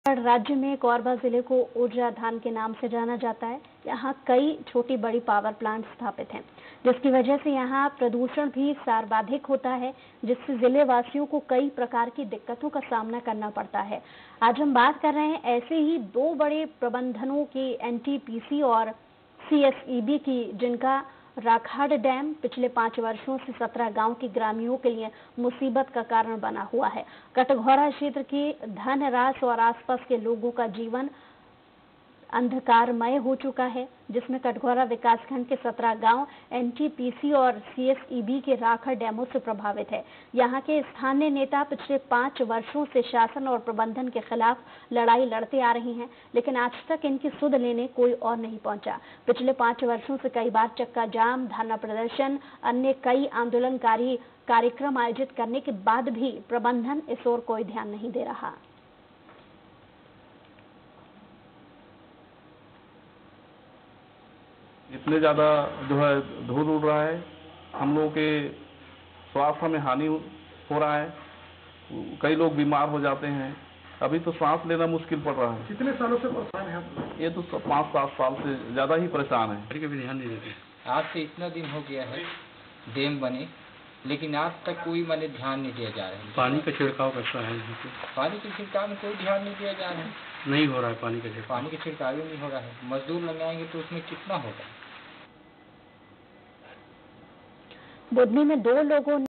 Korba News: NTPC और CSEB के राखड से कोरबा प्रभावित, लोगों का जीना हो रहा दुश्वार
Korba Police Kusmunda, Korba (c.g)